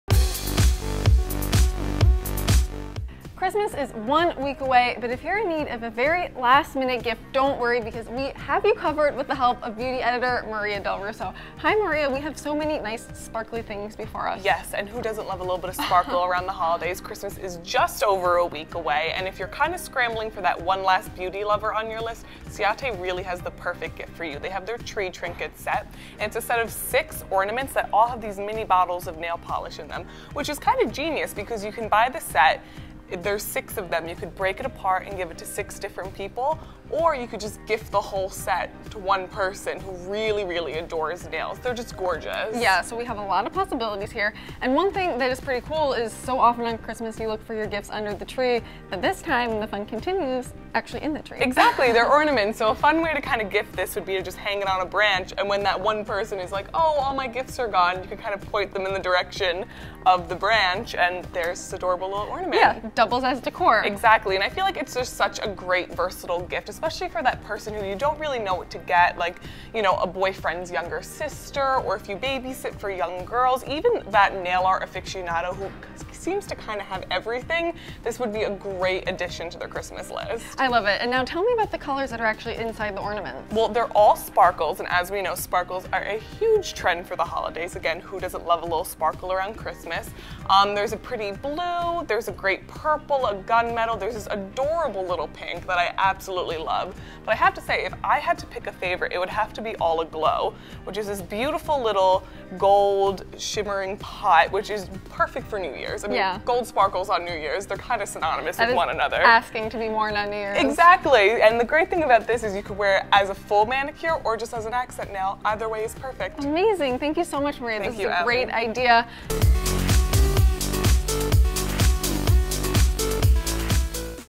Ciate Nail Polish is the Perfect Last-Minute Gift | Beauty Gift Guide
Christmas is just a week away, and if you're like us, there are a couple of people you forgot to add to your gift list. Luckily, Ciaté takes the work out of those tough-to-give gifts with its Tree Trinkets. The box set contains six baubles you can give together or break down to give separately. Find out why this might be the last thing you have to buy before your holiday shopping is done!